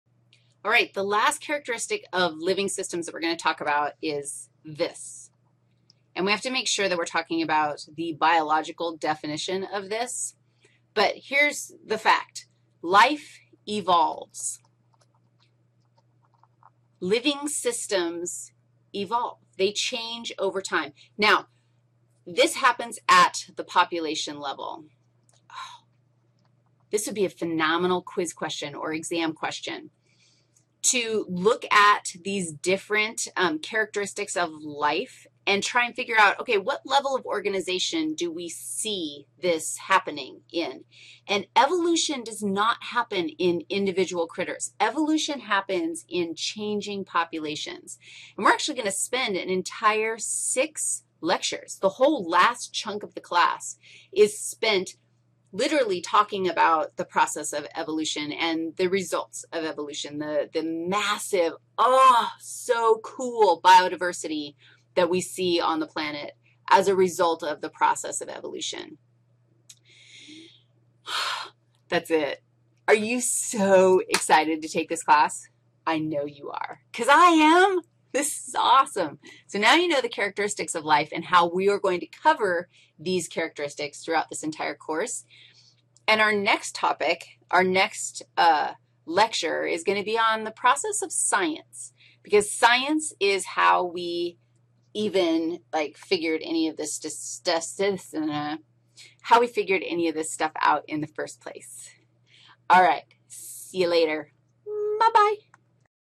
Life 6- Evolution
Part 6 in a 6 part lecture on LIFE in a flipped General Biology course taught by Wendy Riggs. CC-BY. Watch the whole lecture (all 6 videos) by going to the PLAYLIST